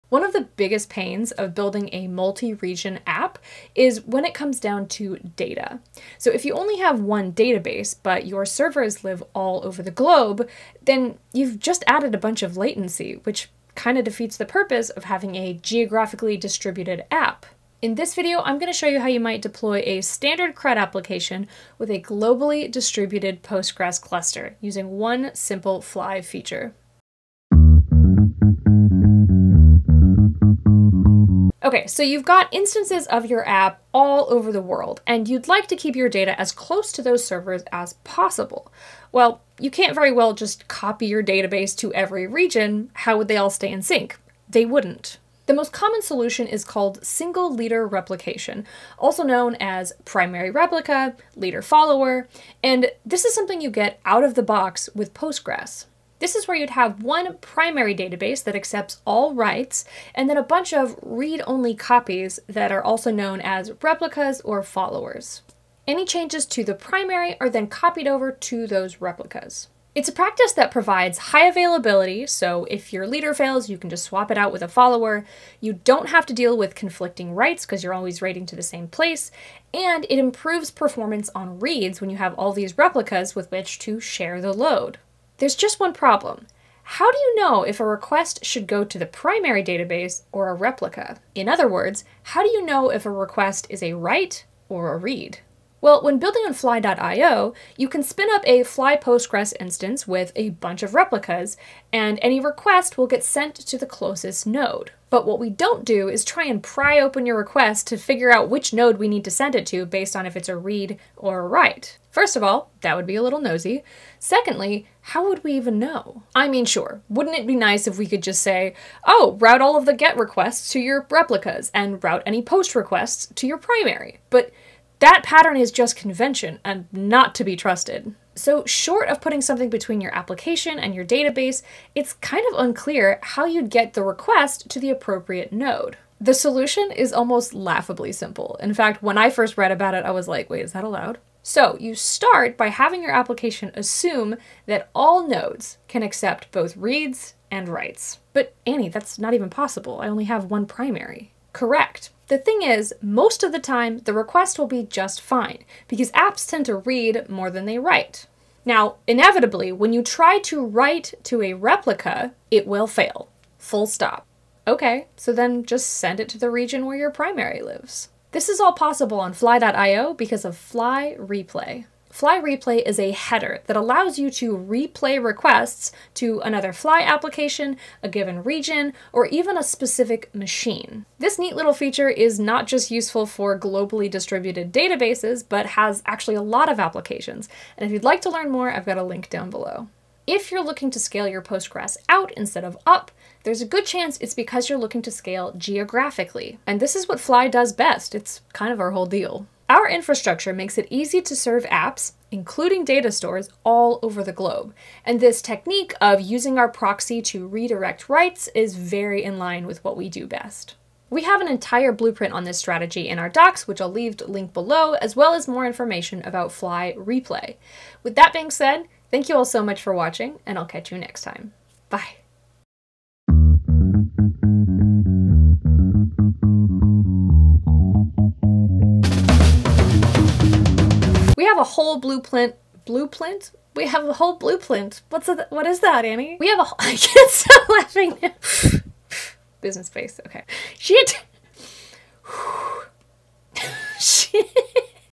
Globally Distributed Postgres on Fly.io | Single leader replication + fly-replay
Deploying your app to multiple regions is great – until you have to make long-distant calls to your ONE database that lives halfway across the world. In this video I'll show you a simple technique that makes it easy to deploy a globally distributed Postgres cluster using a really cool feature on Fly.io called "fly-replay".

Video written and produced by Annie Sexton.